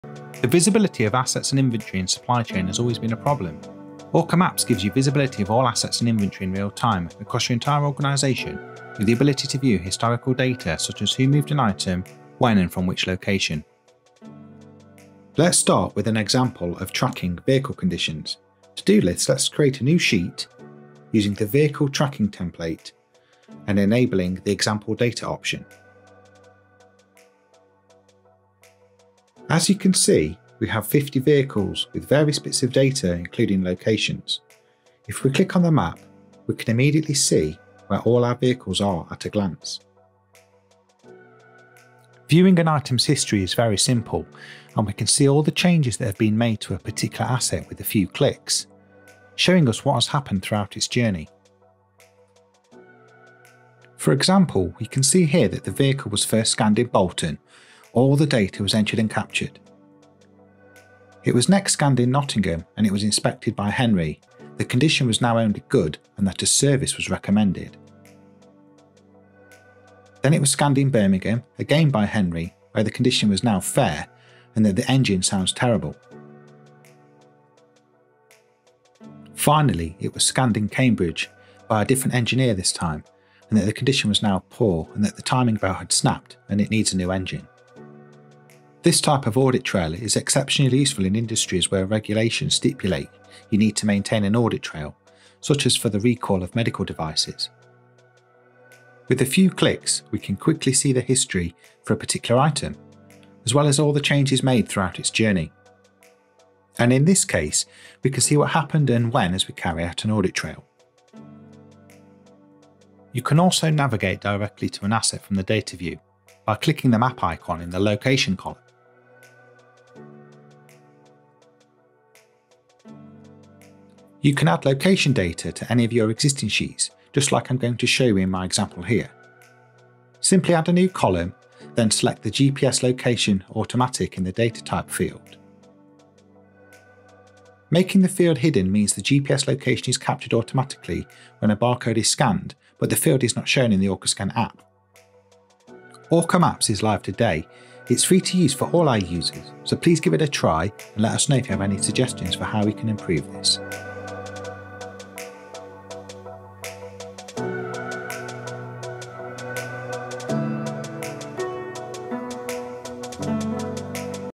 Orca Training - Lesson 5: How to view assets and inventory on a map
How to view assets and inventory on a map using a free account

and log with our Orca Scan account.

Orca Scan makes it easy for companies to build barcodes solutions using smartphones. No coding, special hardware or up front payments required.